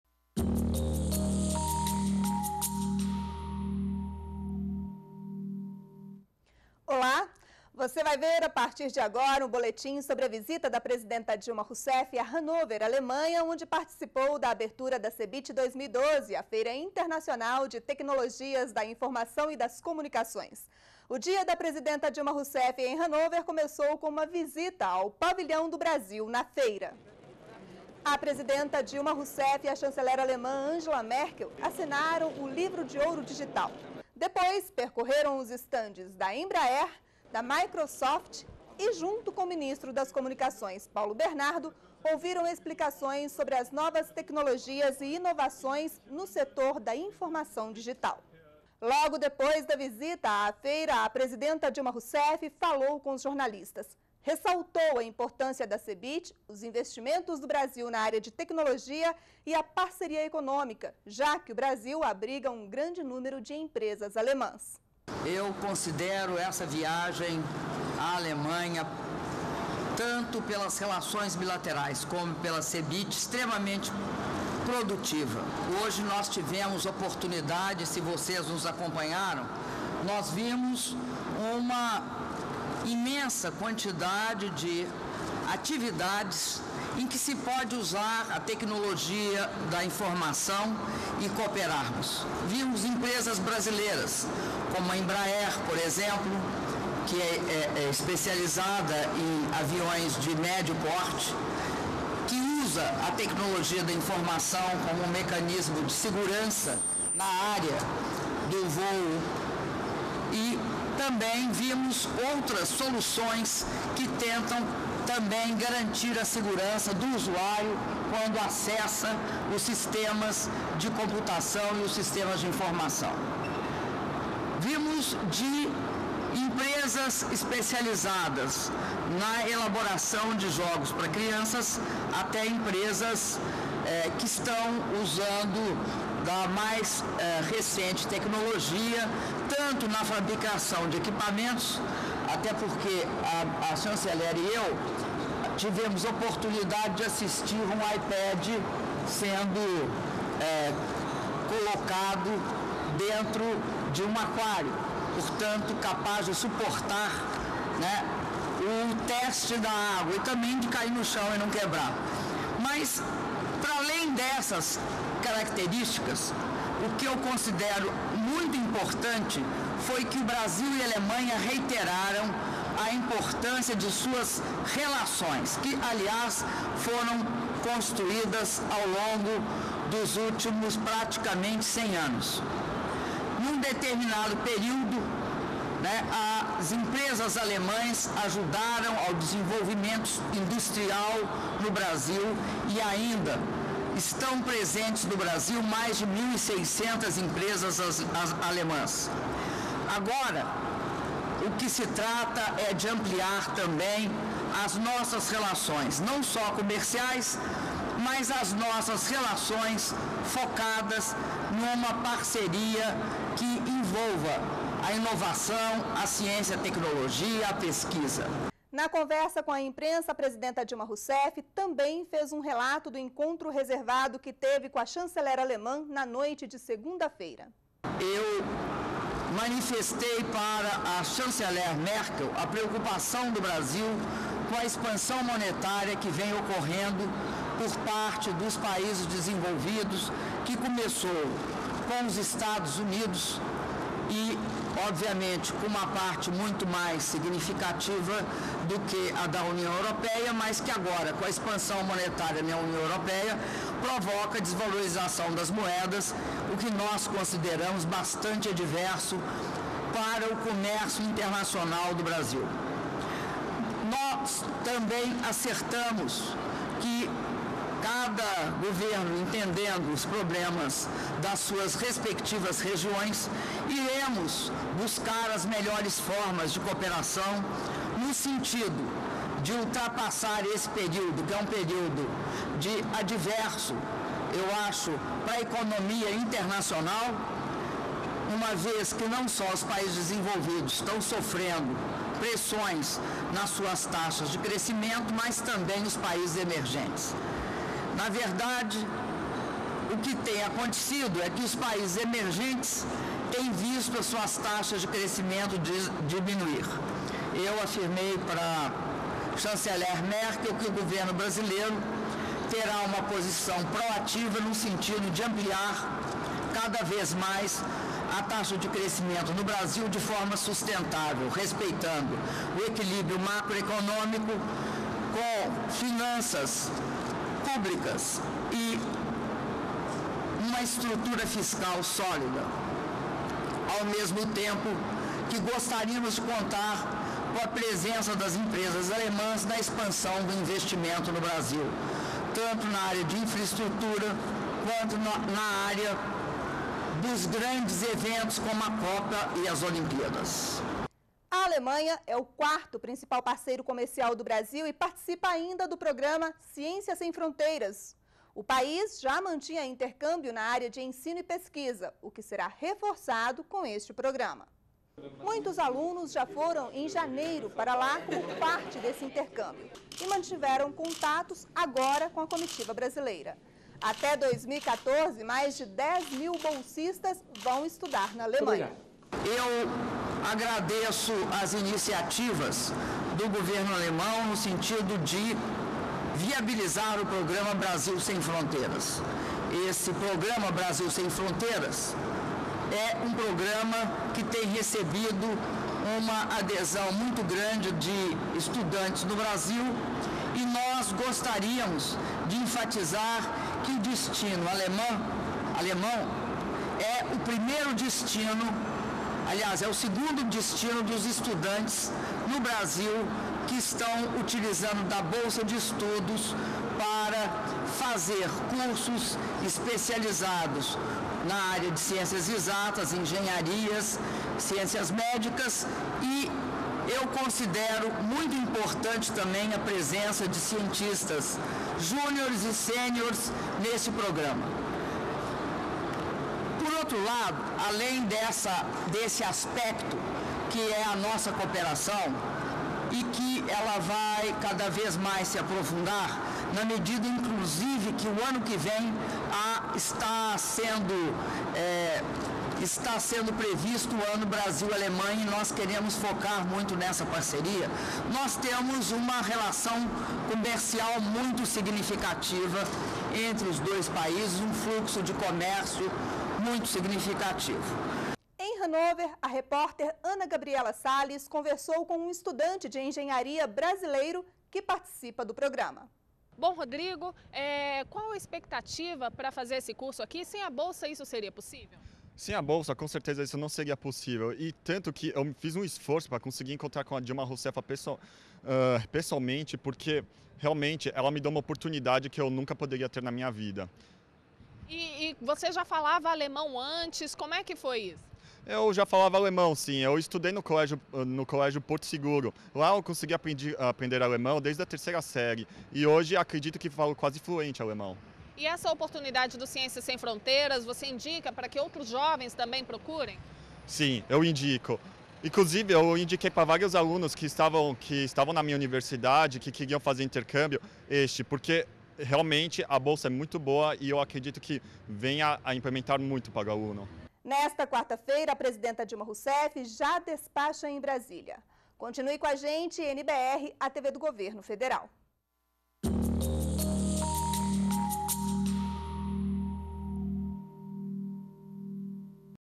Dilma destaca investimentos do Brasil em tecnologia durante visita a CeBIT na Alemanha
ATIVIDADES DA PRESIDENTA - 06.03.12: A presidenta Dilma Rousseff visitou Hannover, na Alemanha, onde participou da abertura da Cebit 2012, a Feira Internacional de Tecnologias da Informação e das Comunicações. O dia da presidenta em Hannover começou com uma visita  ao pavilhão do Brasil na Feira. A presidenta Dilma e a chanceler alemã Angela Merkel assinaram o livro de ouro digital. Depois percorreram os stands da embrer, da microsoft e, junto, com o ministro das Comunicações, Paulo Bernardo, ouviram as explicações sobre as novas tecnologias e inovações no setor da informação digital. Logo depois da visita à feira, Dilma Rousseff falou com os jornalistas e ressaltou a importância da Cebit, os investimentos do Brasil na área de tecnologia e a parceria econômica, já que o Brasil abriga um grande número de empresas alemãs.